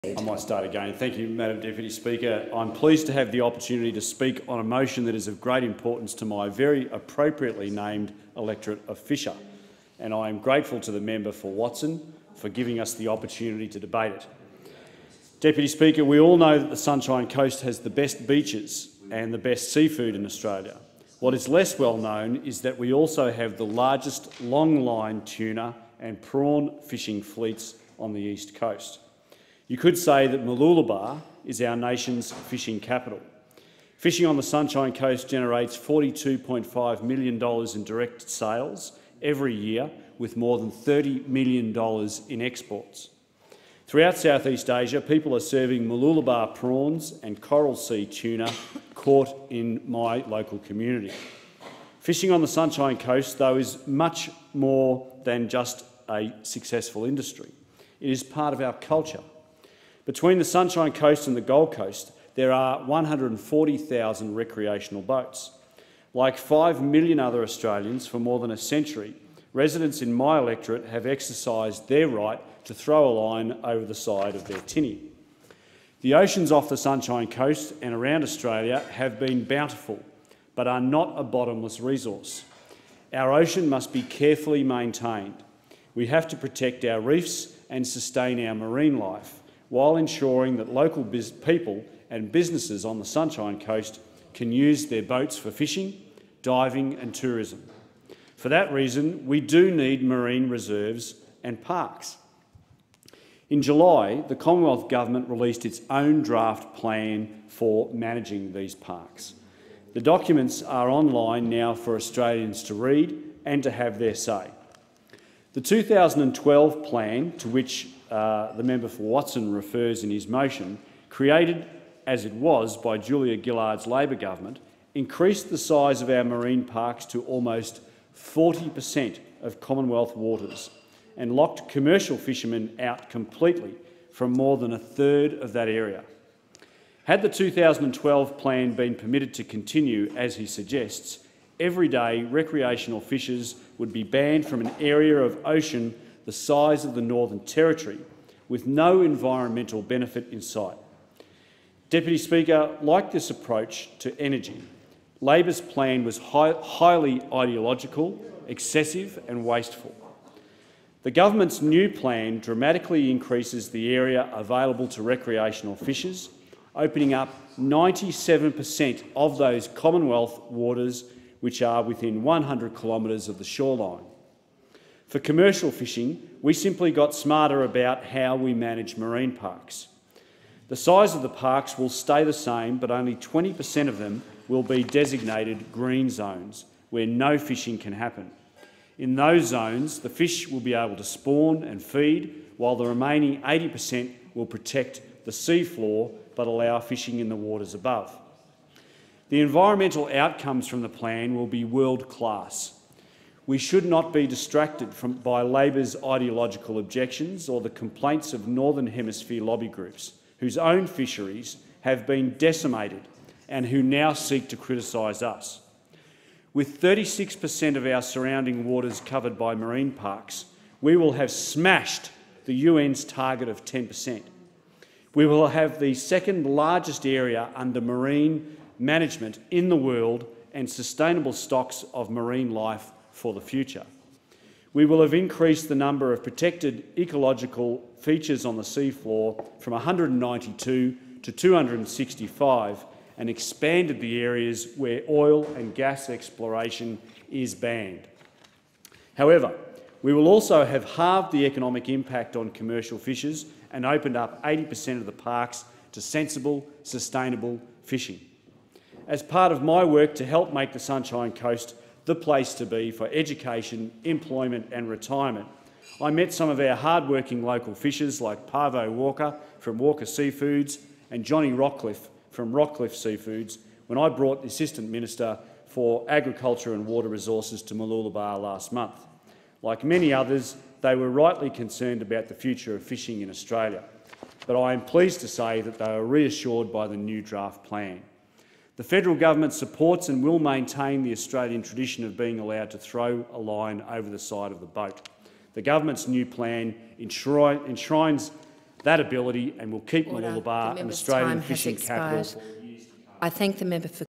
Andrew Wallace MP on Marine Park Conservation
Speech in the Federation Chamber 14 August 2017.